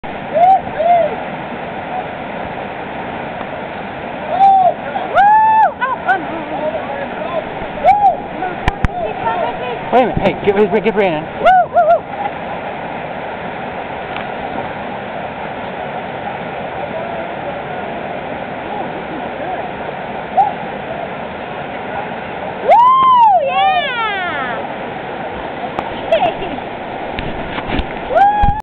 Nantahala Falls in Bulldog Canoe from compositecreations.ca
This is Brandon Carr Running The Nantahala Lesser Wesser Falls in a Bulldog Canoe from Composite Creations using the right side line. compositecreations.ca